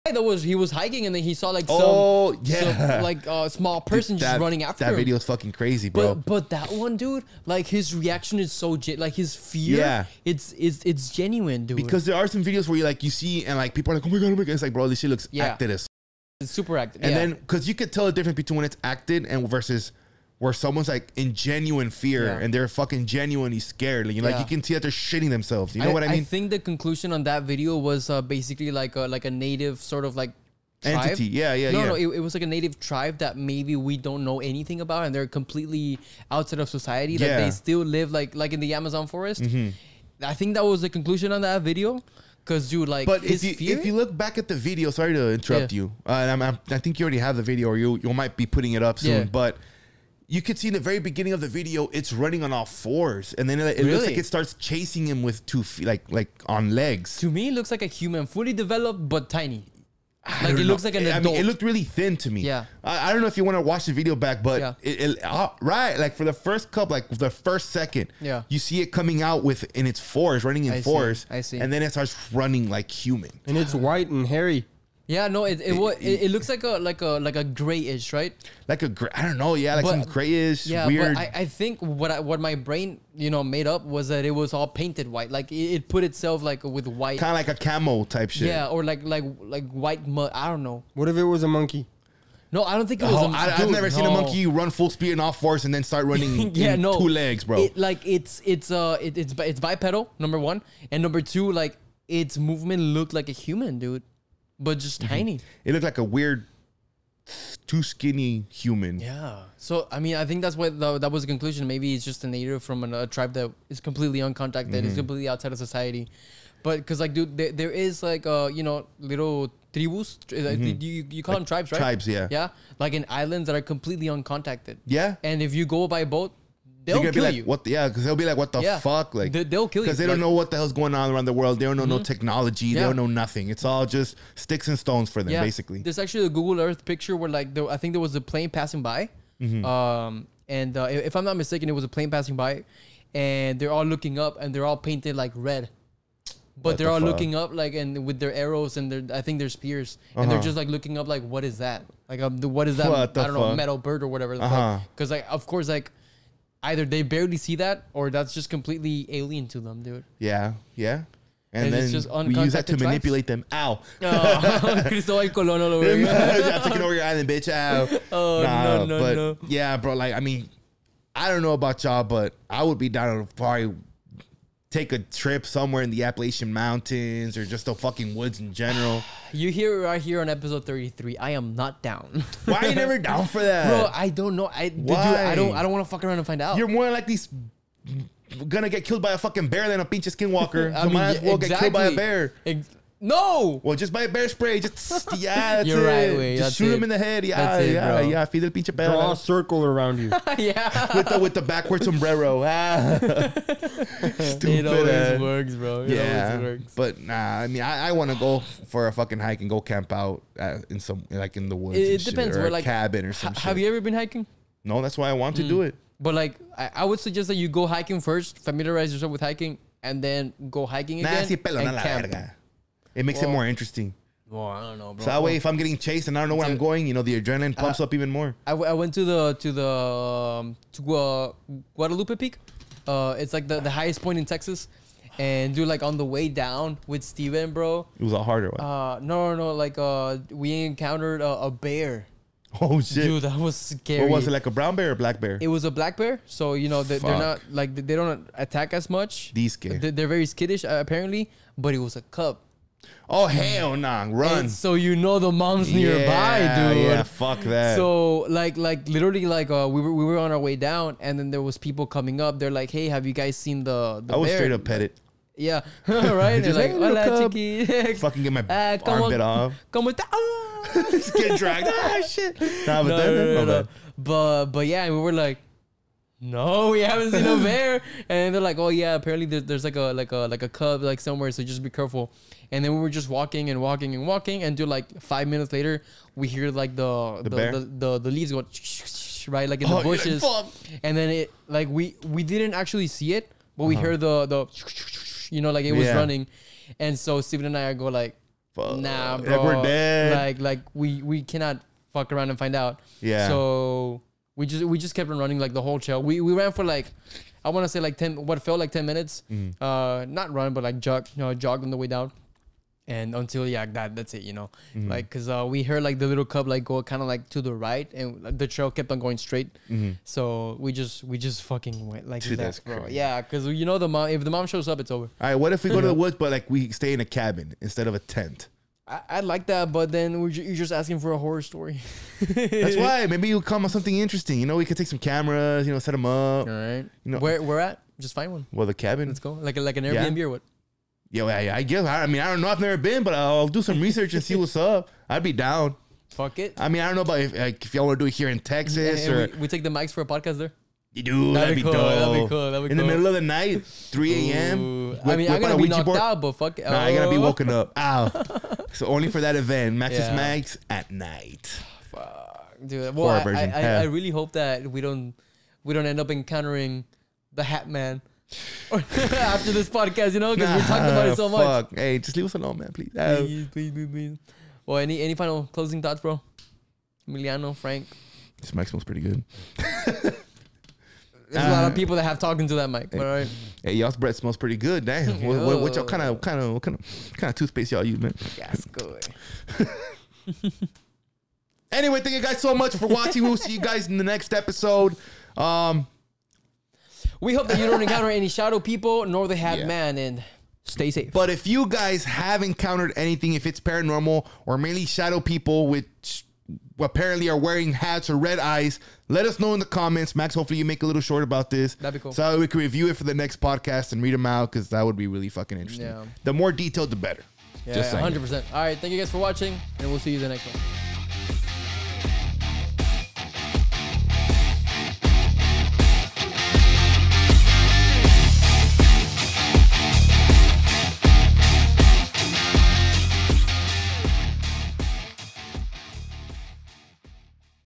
Man Chased in Forest by Unknown Tribe Member
Max

Youtube: You know this one

In this clip of "Max's Mics," we delve into a tale of a mysterious chase in the depths of a secluded forest. Join us as we explore the unsettling encounter with a humanoid bipedal entity.

While venturing through a dense forest, a man experienced the terror of being pursued by what appeared to be an uncontacted native or an unknown creature. This eerie humanoid, moving swiftly on two legs, chased him through the underbrush, raising questions about its origins and intentions.

Watch as we analyze the details of this chilling pursuit and discuss various theories about the entity. Could it have been an undiscovered native, or something far more mysterious?